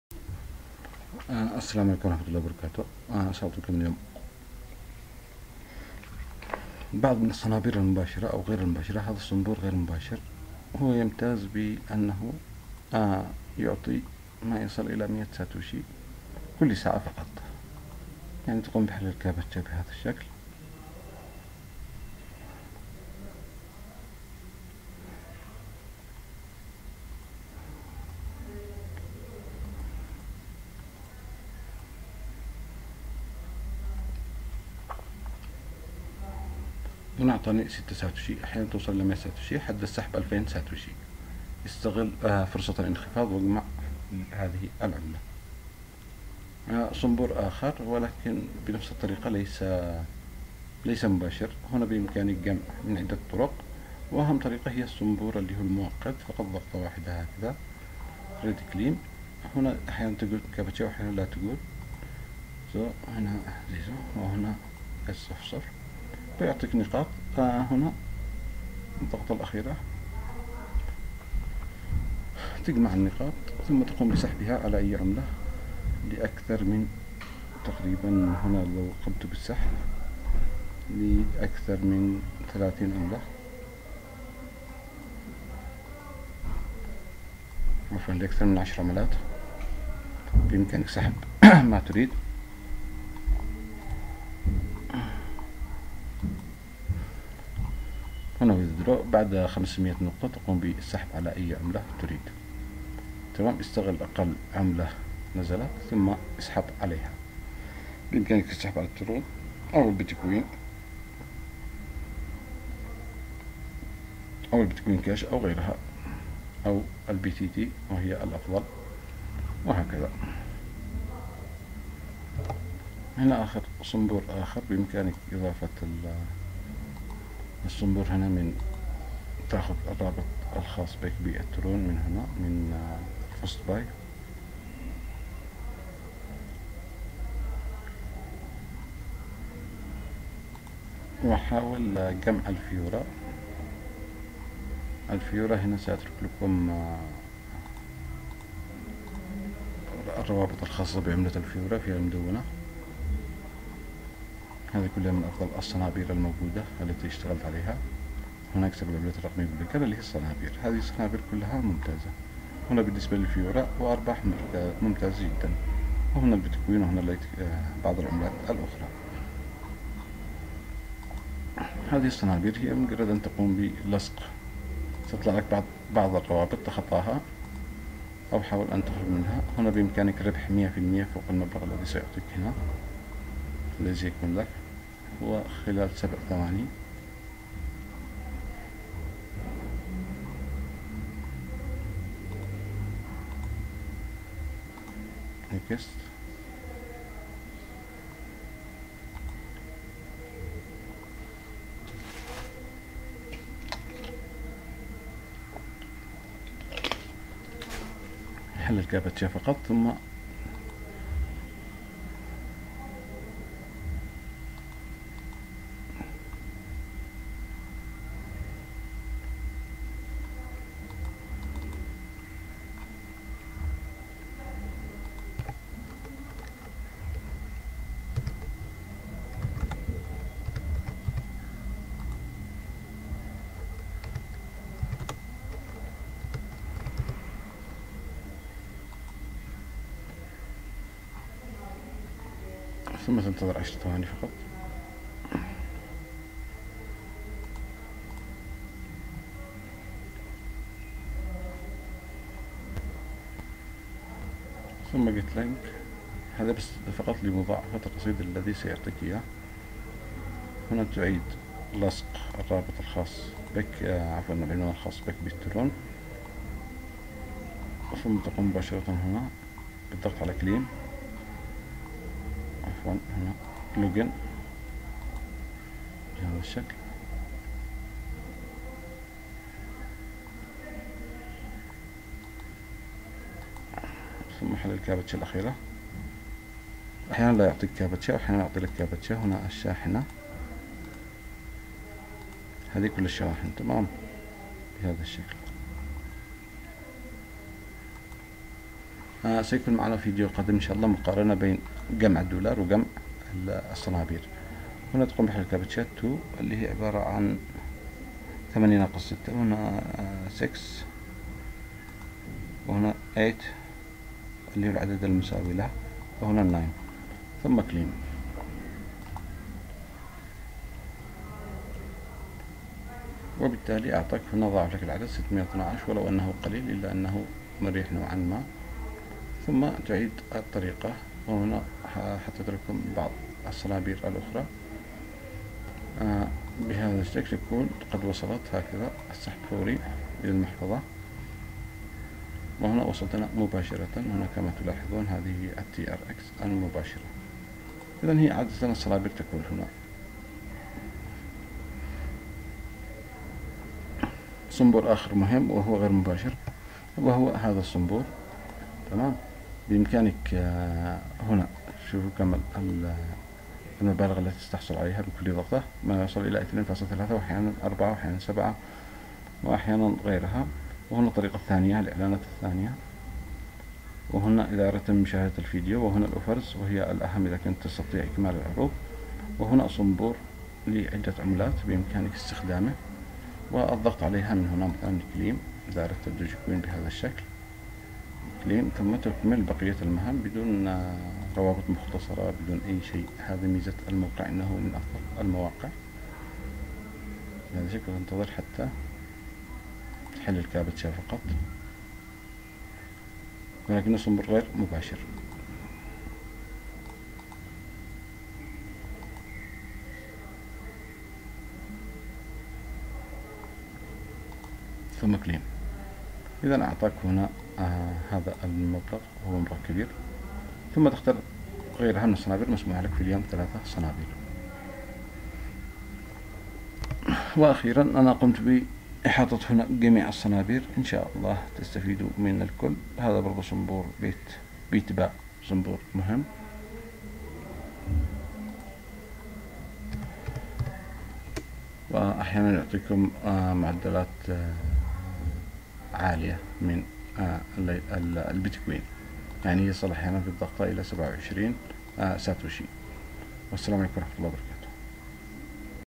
اكسب العملات الرقمية من افضل المواقع للمبتدئين
اجمع البتكوين كل ساعة بضغطة واحدة حد السحب 2000 ساتوشي

عملة الترون  TRON (TRX)
اجمع الترون TRX
المواقع مباشرة والسحب فوري للمحفظة
الموقع